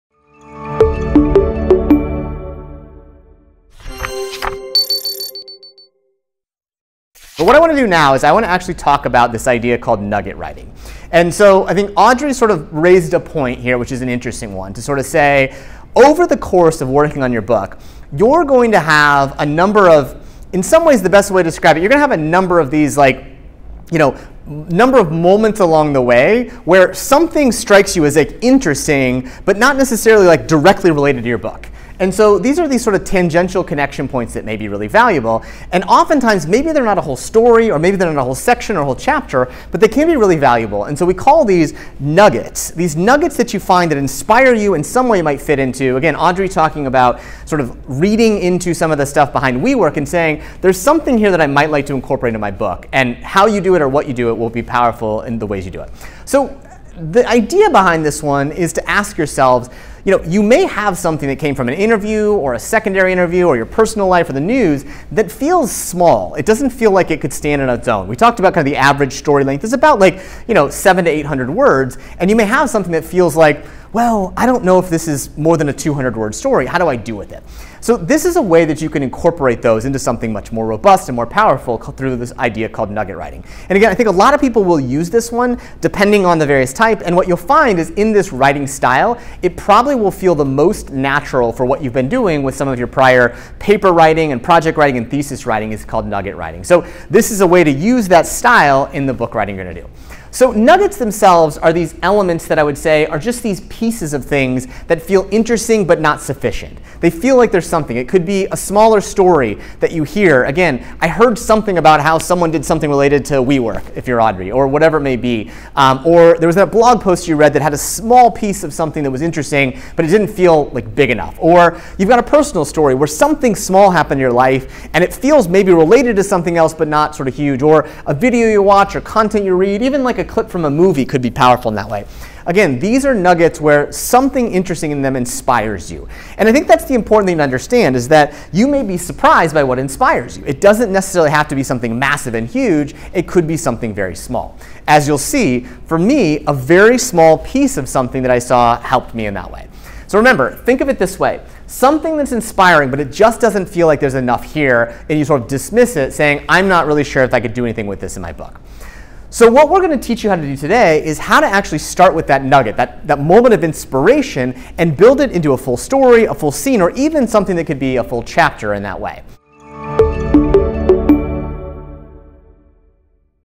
What is Nugget Writing || Nugget Writing for Nonfiction Books
In this video, we'll go a little deeper into understanding nugget writing. Nuggets are the elements in writing that feel interesting, but not sufficient. They simply inspire you, but they're not enough to be as a stand-alone story.

Nugget writing could be a smaller story that you can incorporate in your bigger idea, a book chapter you started.